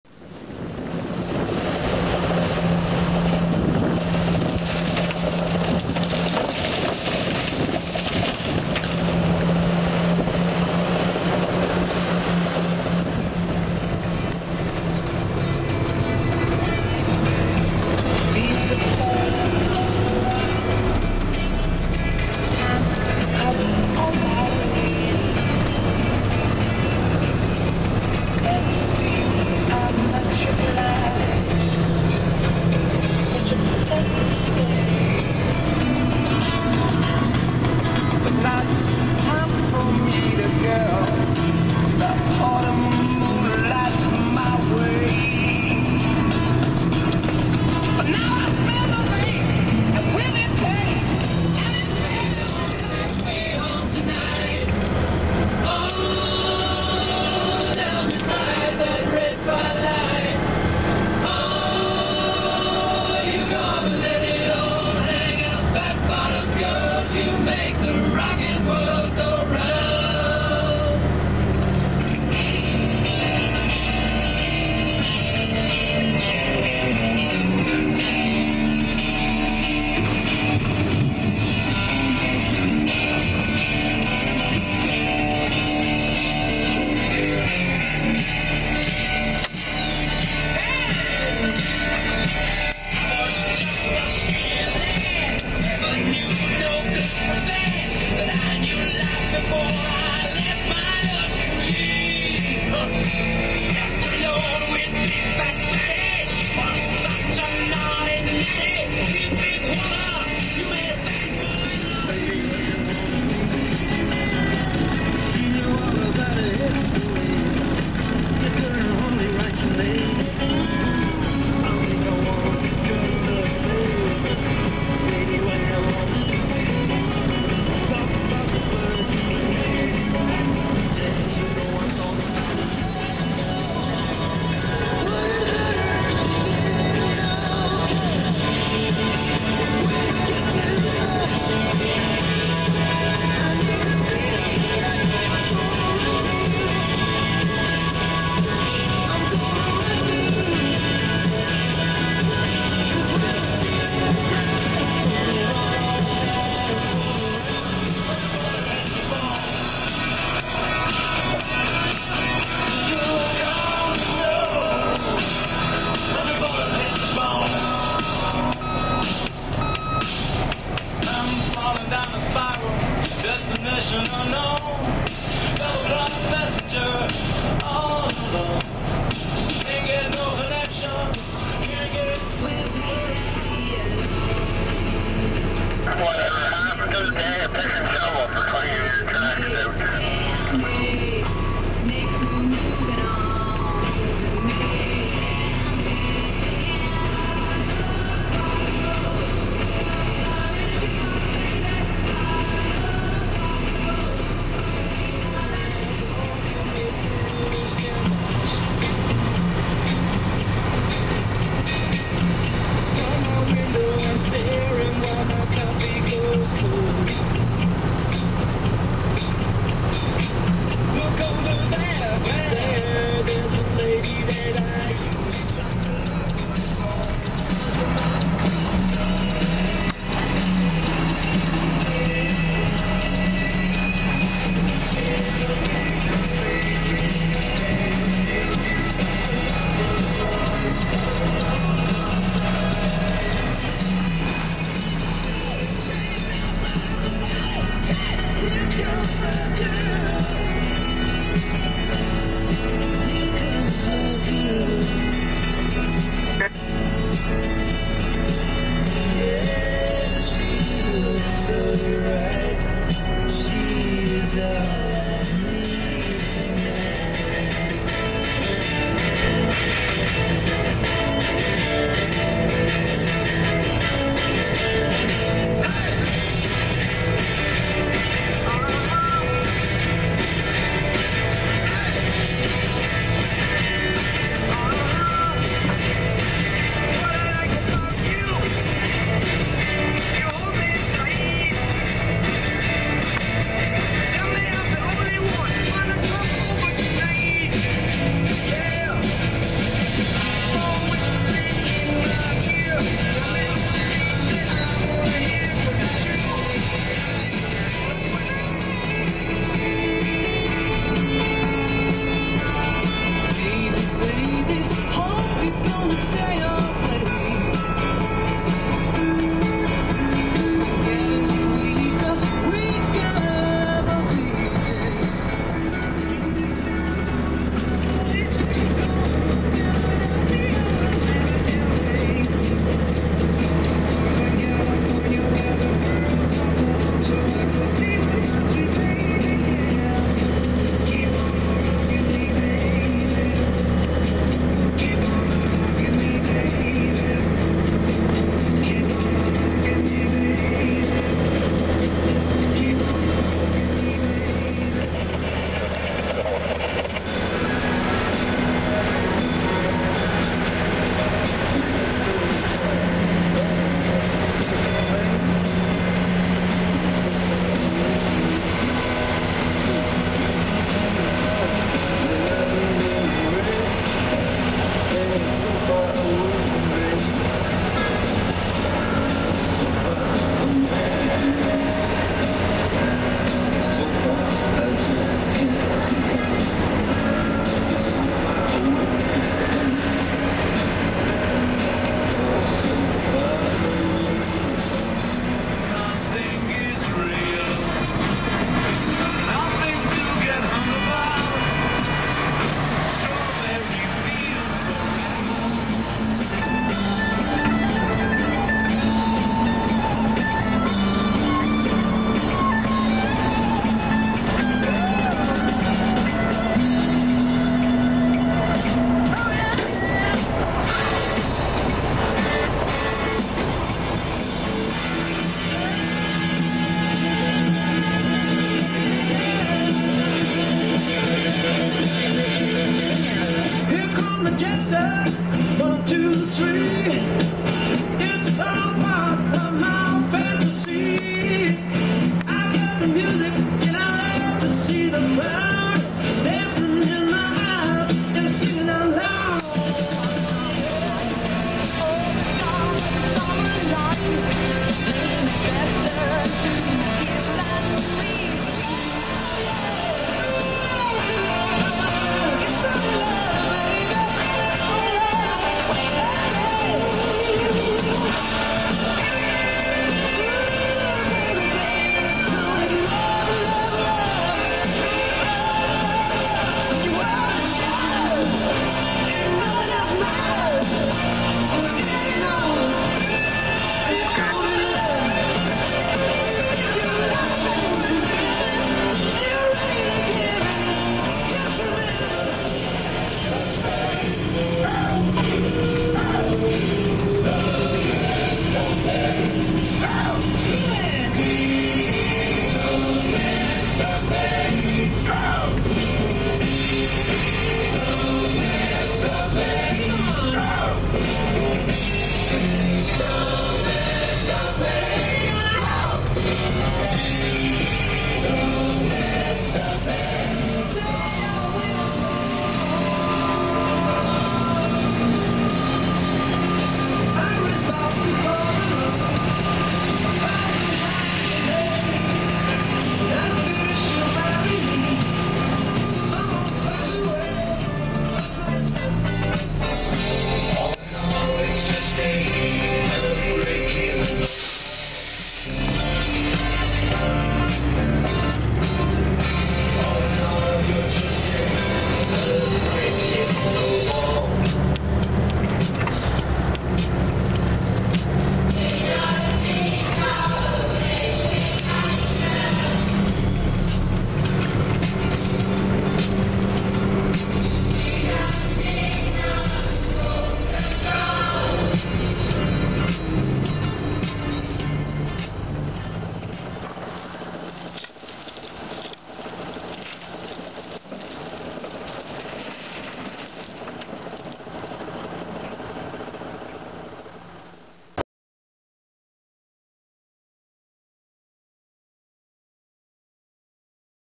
Rock n Roll n Rock Trucks
Listening to Rock n Roll on Sirius Radio while trucking at the stripmine. Rock n Roll and heavy metal...what a combo:)